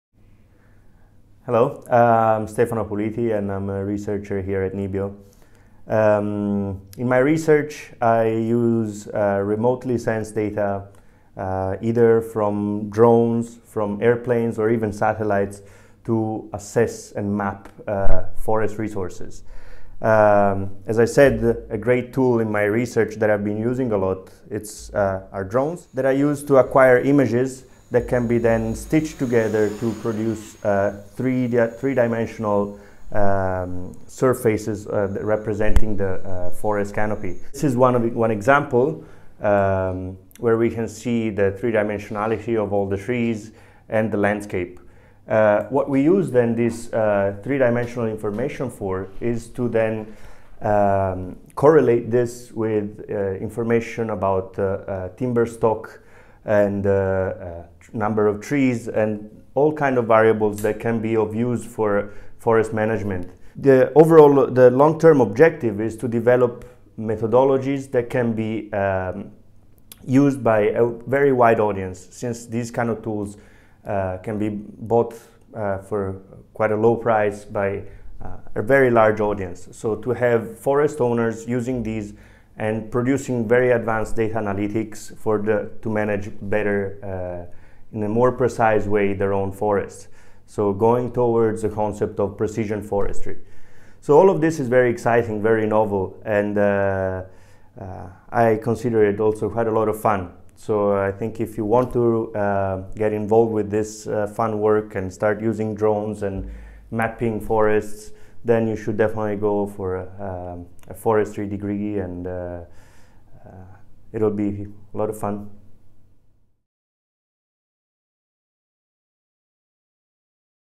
Skogforskning med droner
Stefano Puliti er skogforsker hos NIBIO, droner er et av de viktigste arbeidsverktøyene.